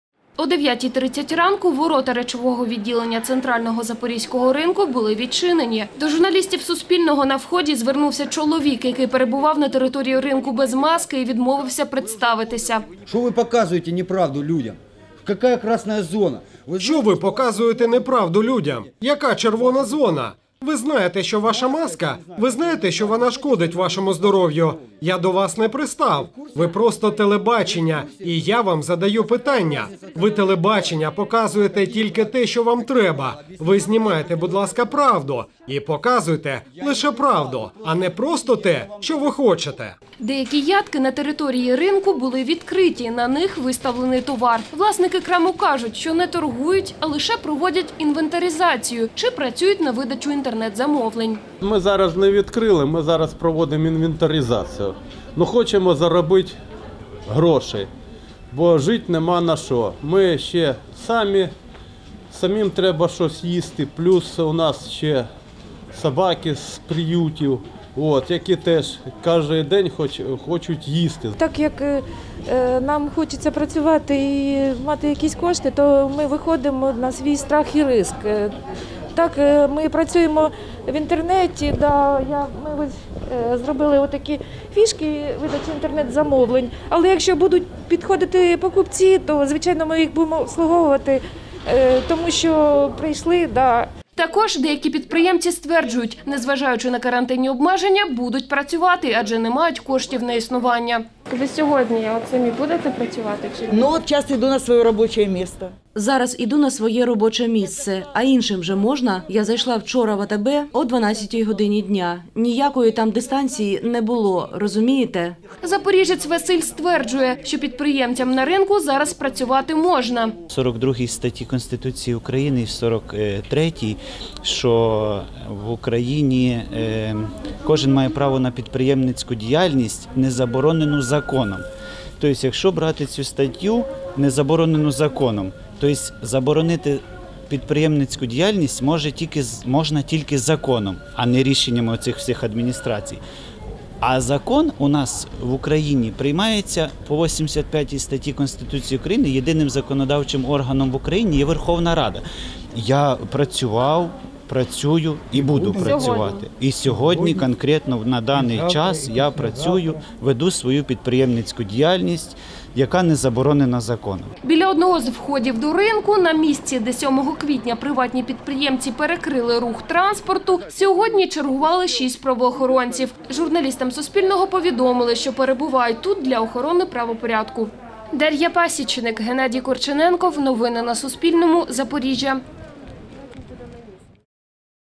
«Червона» зона: як працює Центральний ринок Запоріжжя | Новини
У Запоріжжі введений «червоний» рівень епідемічної небезпеки. У місті діють додаткові карантинні обмеження. Зокрема заборонено діяльність ринків, крім продовольчих. Як дотримуються карантинних обмежень на Центральному ринку Запоріжжя далі у сюжеті Суспільного.